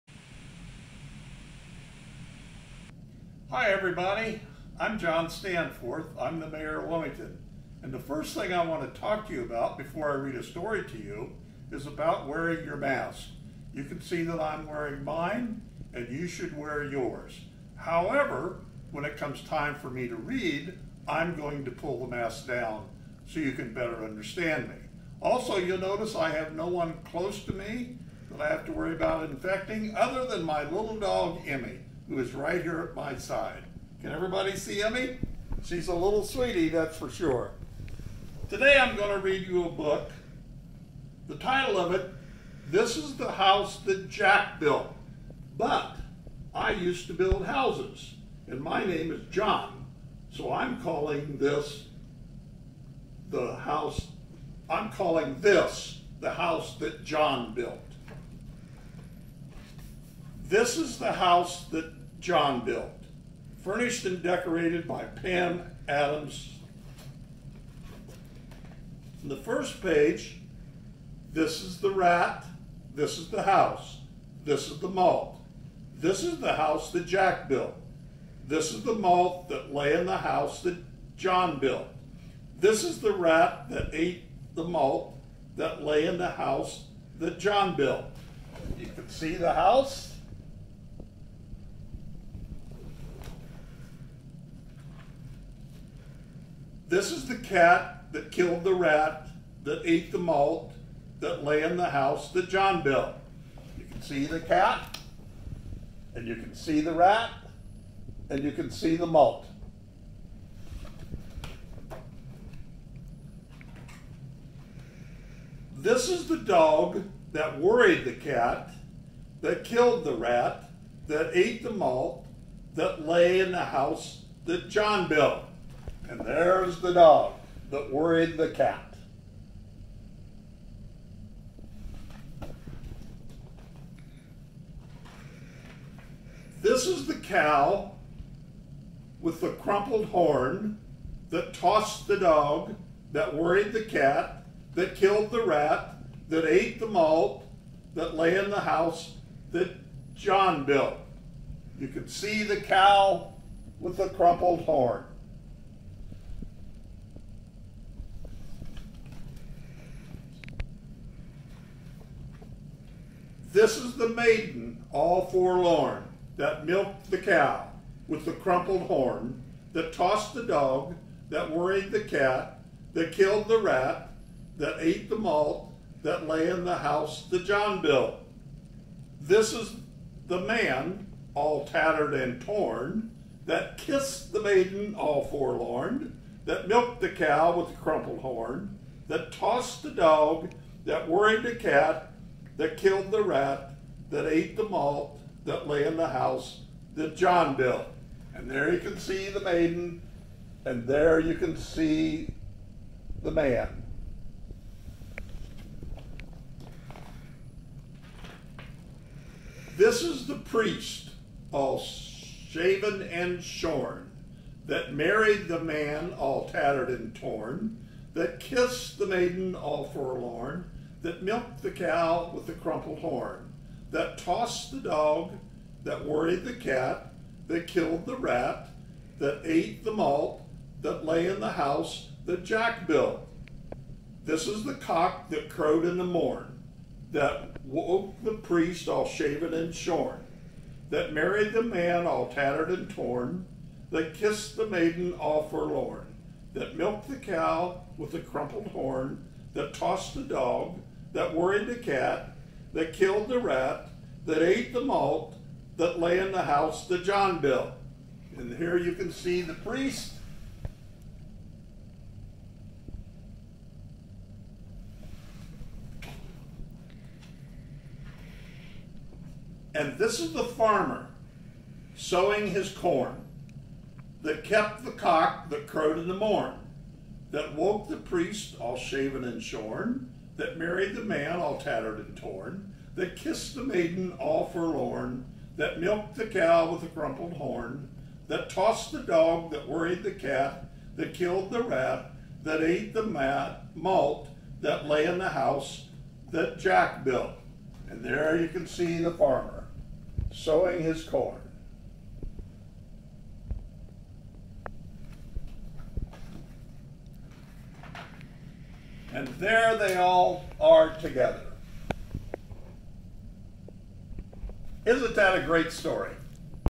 Kindergarten- Week 1- This is the House that Jack Built
Written by Pam Adams
Read by John Stanforth